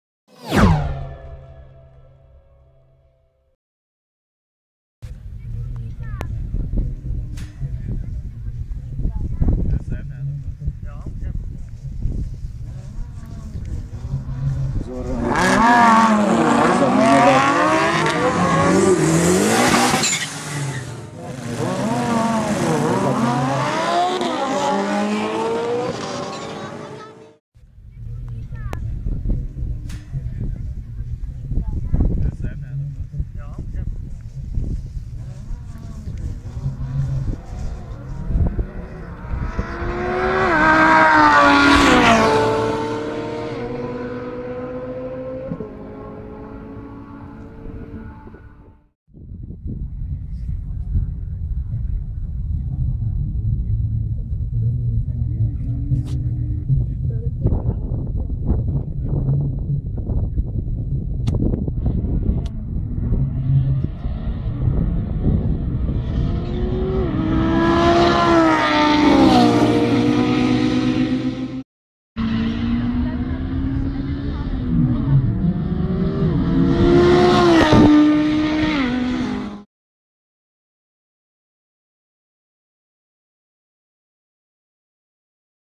Lamborghini Huracan Vs Ferrari 458 Italia Drag Race - DRAGINFO
Lamborghini Huracan Vs Ferrari 458 Italia Drag Race Video!
This was filmed at the Drag Day in Hoskovice in Czech Republic.
Which one do you prefer: Huracan LP610-4 or 458 Italia?